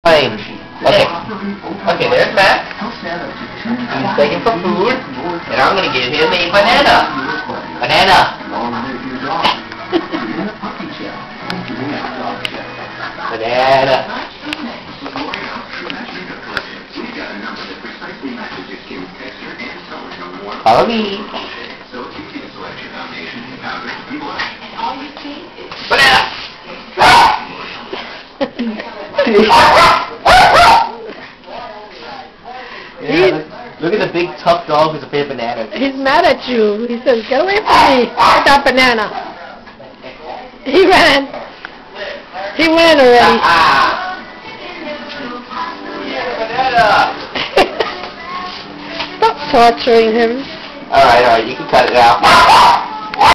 Max vs banana
my dog Max is always begging for table scraps, yet he is literally afraid of bananas...I don't know why